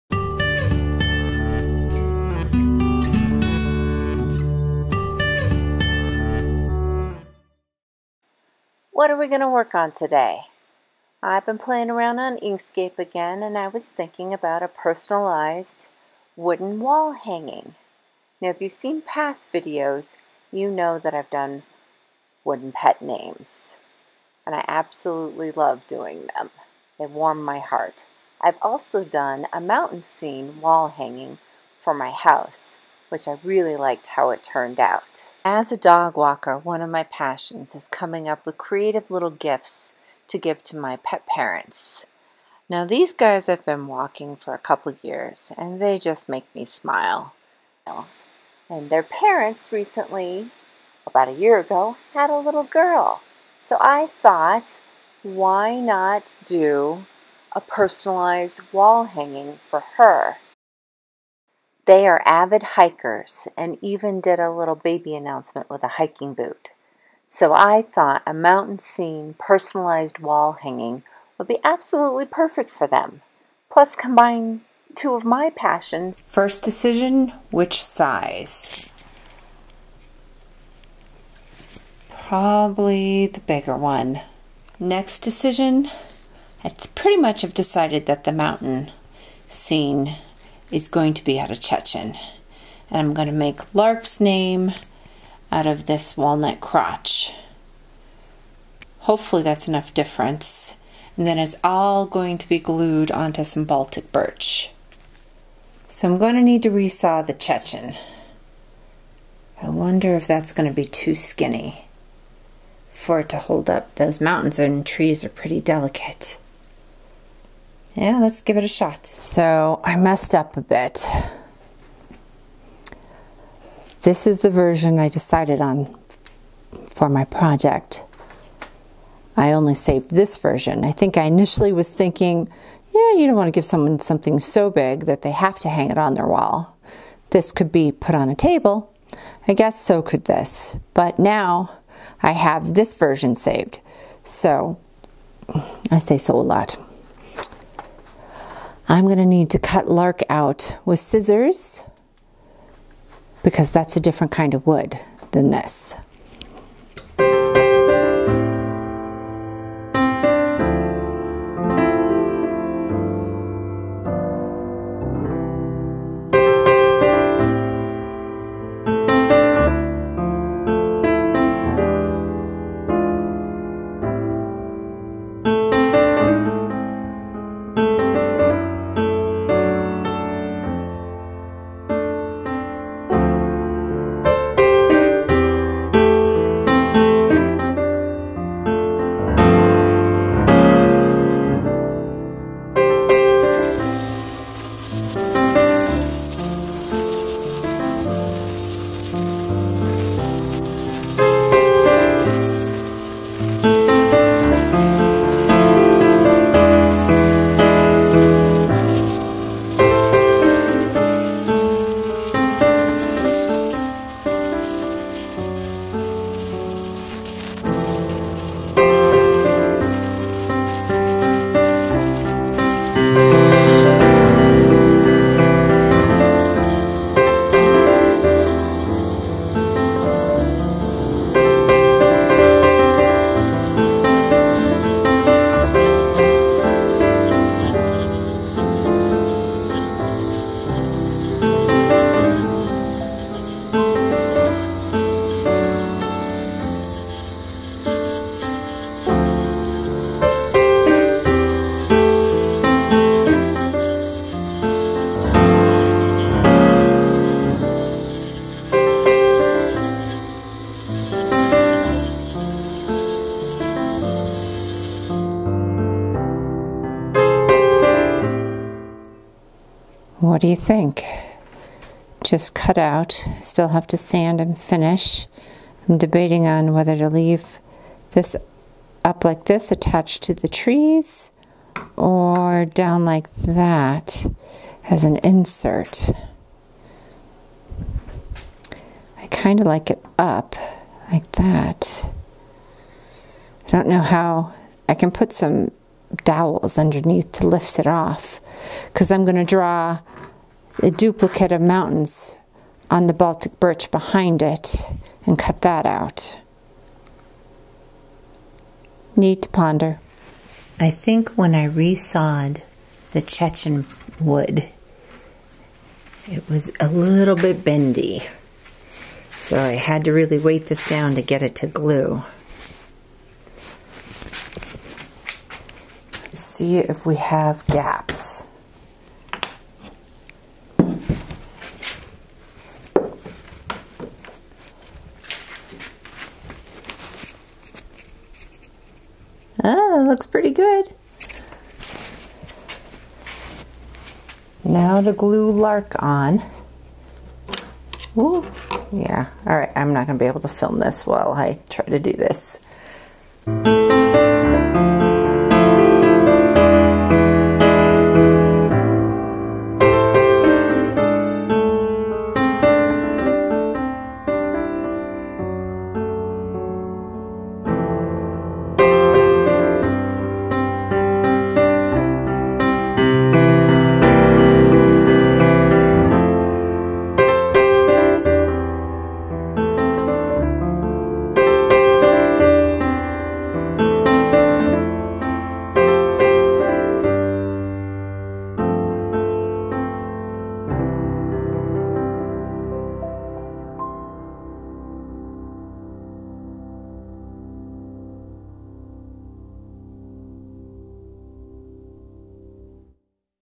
Making a Personalized Wooden Wall Hanging
This video is about Making a Personalized Wooden Wall Hanging on scroll saw.